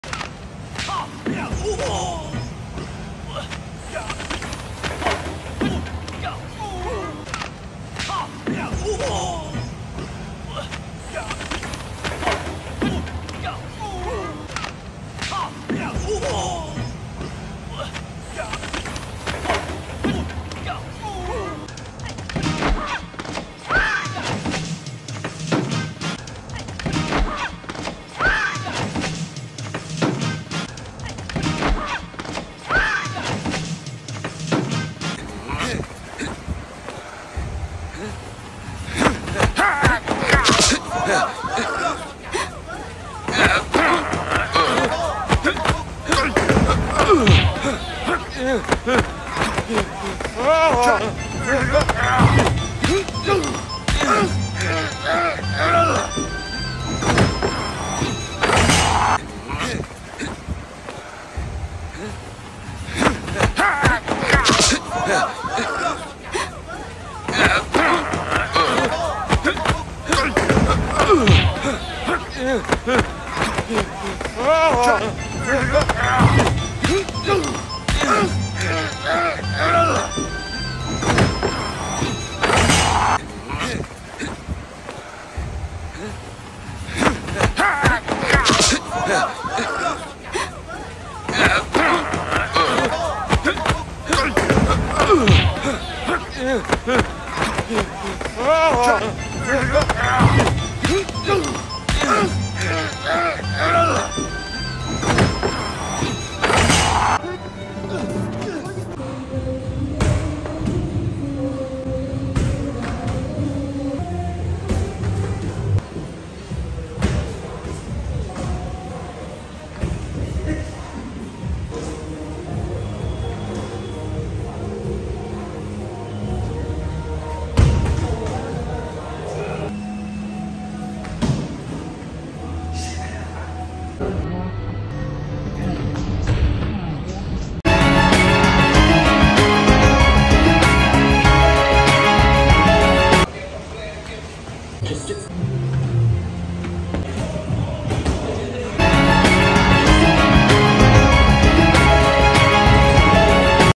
RECREATING ICONIC JACKIE CHAN STUNTS
Welcome to my ultimate tribute to the
legendary Jackie Chan. In this video, we've recreated some of Jackie Chan's most iconic
movie stunts, bringing his epic action scenes to life. Whether you're a fan of Jackie Chan's martial arts prowess or his incredible stunt work, this video is for you!